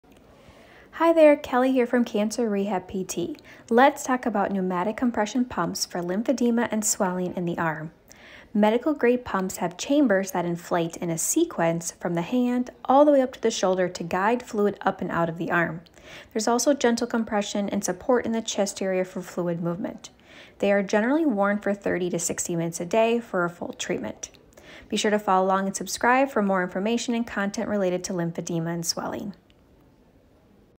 Pneumatic Compression Device for Lymphedema in the Arm
Pneumatic Compression Pumps or devices for arm lymphedema

*The product and some media used in this video were provided by Airos Medical. Thank you to Airos Medical!